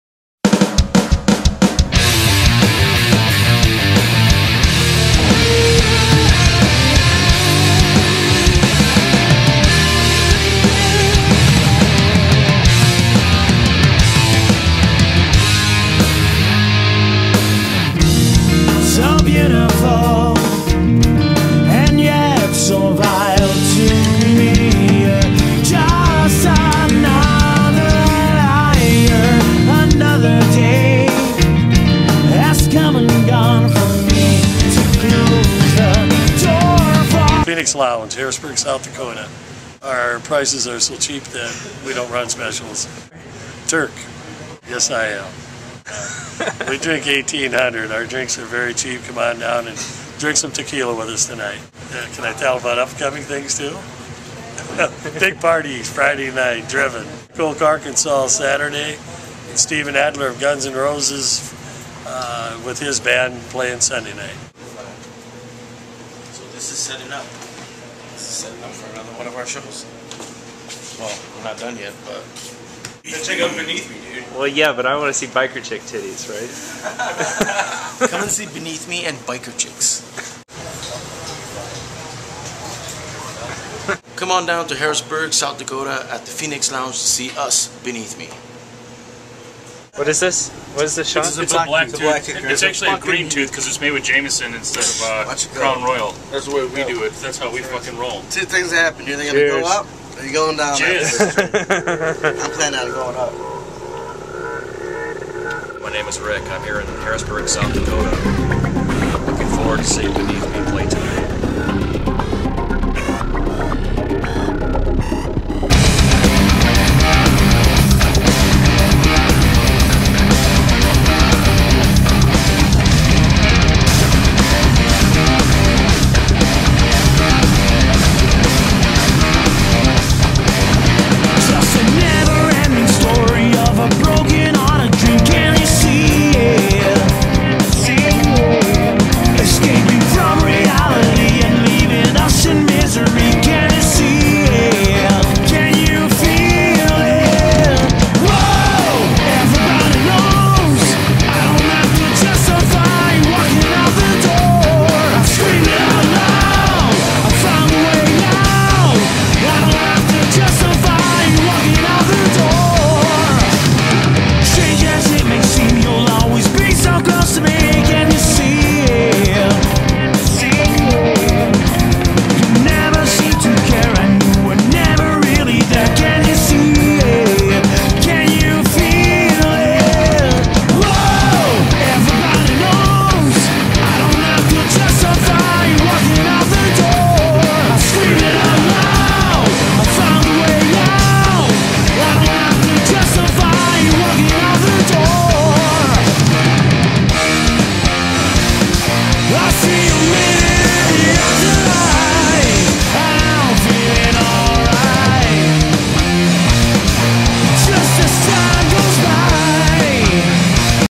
On The Road With Beneath Me: Sturgis: Day 2
We are almost to Sturgis and Bike Week and crazy biker fun, but we stopped to play a gig at the Phoenix Lounge in Harrisburg, South Dakota. Turk the owner is a real cool dude and their sound system could probably kill someone. Beneath Me killed it, but really this was just a warm up for gigs this weekend.

I wish my camera had a real mic on it and I could capture the full live experience.